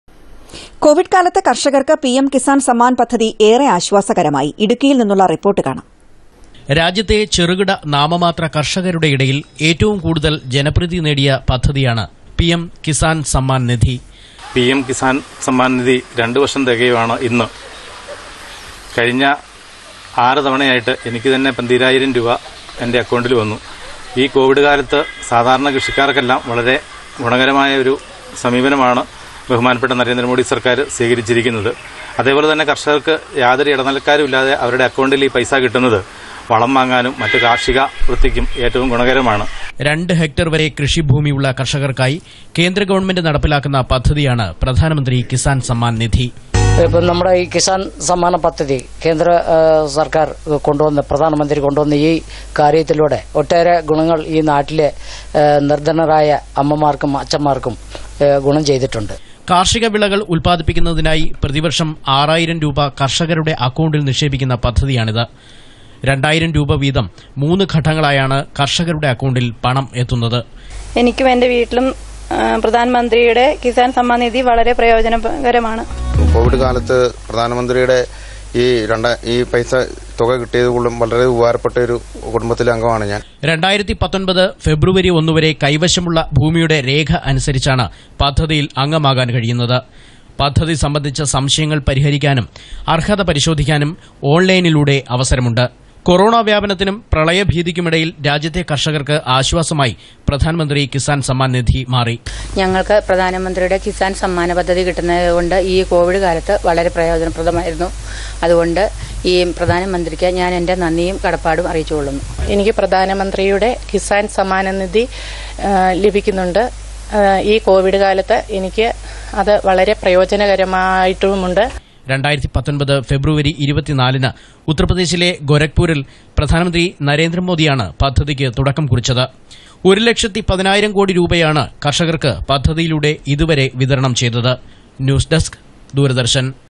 പി.എം കിസ്സാൻ സമ്മാൻ നിധി രണ്ടു വർഷം പൂർത്തിയാകുമ്പോൾ ഇടുക്കിയിലെ കർഷകർ സന്തോഷത്തിലാണ്
കേന്ദ്ര സർക്കാർ പി.എം കിസ്സാൻ സമ്മാൻ പദ്ധതി ആവിഷ്ക്കരിച്ചിട്ട്  ഇന്ന് രണ്ടു വർഷം പൂർത്തിയായി.കാർഷികഗ്രാമങ്ങളിലെ ആയിരക്കണക്കിനു കർഷകർക്ക് പ്രതിവർഷം ആറായിരം രൂപ ലഭിക്കുന്ന പദ്ധതിയാണിത്.കോവിഡ് കാലത്ത് കർഷകർക്ക് പദ്ധതി ഏറെ ആശ്വാസകരമായി.
2,000 രൂപ വീതം മൂന്ന് ഘട്ടങ്ങൾ ആയാണ് പണം അക്കൗണ്ടിൽ എത്തുന്നത്.2019 ഫെബ്രുവരി ഒന്നു വരെ കൈവശമുള്ള ഭൂമിയുടെ രേഖ അനുസരിച്ചാണ് പദ്ധതിയിൽ അംഗമാകാൻ ആകുക.പദ്ധതി സംബന്ധിച്ച സംശയങ്ങൾ പരിഹരിയ്ക്കാനും അർഹത പരിശോധിയ്ക്കാനും  ഓൺലൈനിലൂടെ അവസരമുണ്ട്. കൊറോണ വ്യാപനത്തിനും പ്രളയഭീതിക്കുമിടയില്‍ രാജ്യത്തെ കര്‍ഷകര്‍ക്ക് ആശ്വാസമായി പ്രധാന്‍മന്ത്രി കിസാന്‍ സമ്മാന്‍ നിധി മാറി.